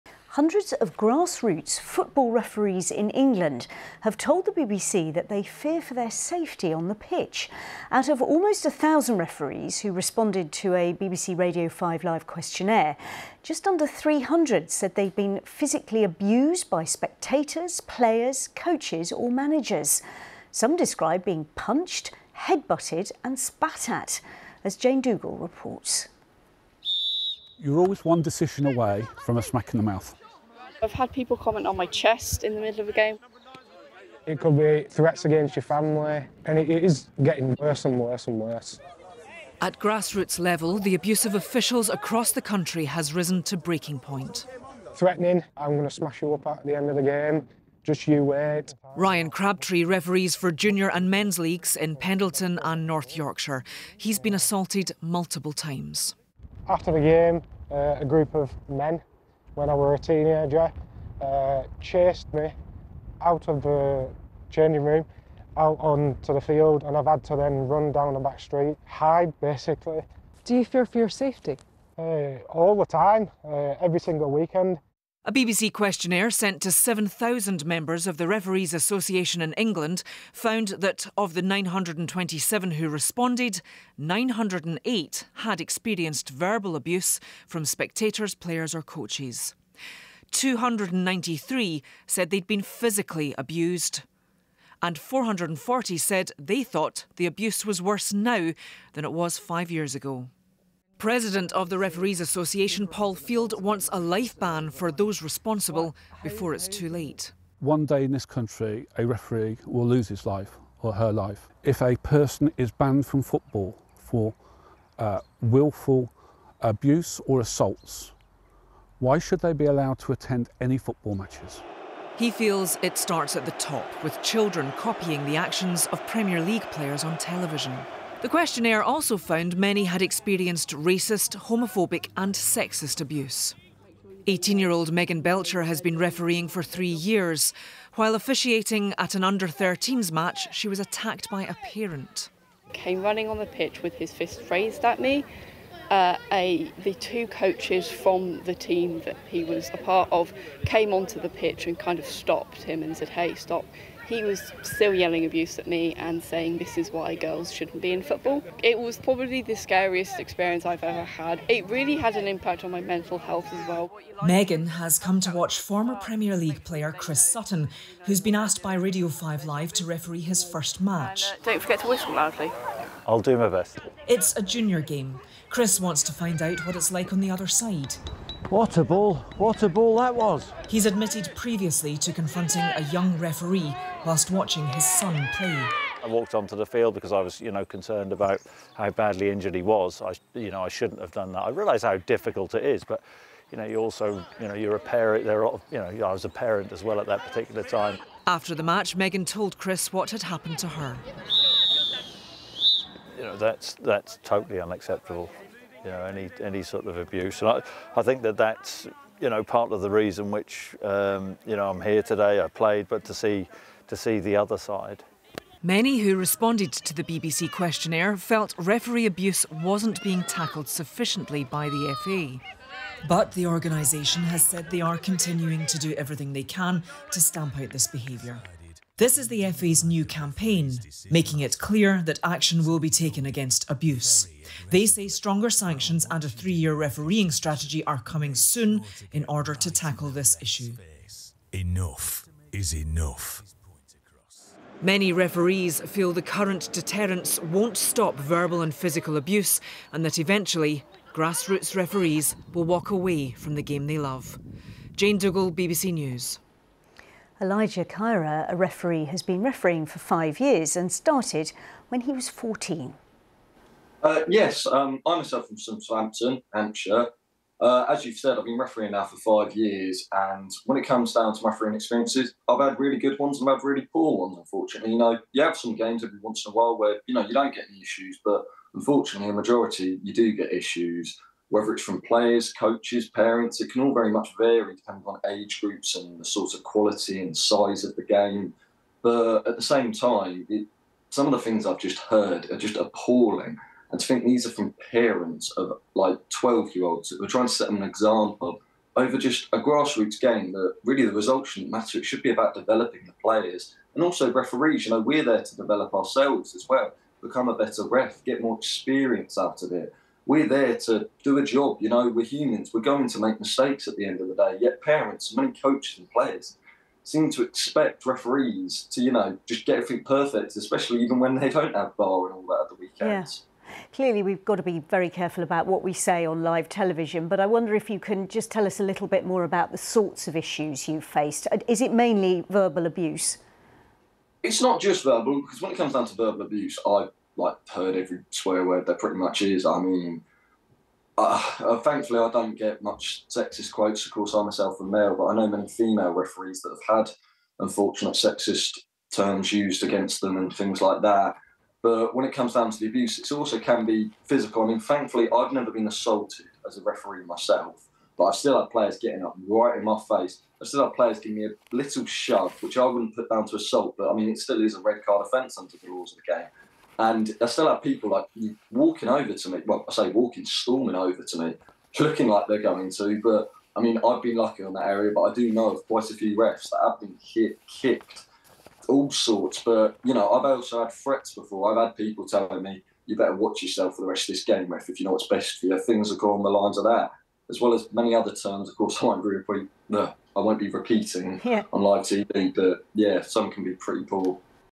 Football referees tell of safety fears at grassroots level - BBC News
Hundreds of grassroots referees have told the BBC they fear for their safety when refereeing and are dissatisfied with current measures to tackle abuse.

More than 900 referees in England responded to a Radio 5 Live questionnaire, with 293 saying they had been physically abused by spectators, players, coaches or managers.

Almost all the respondents had experienced some form of verbal abuse.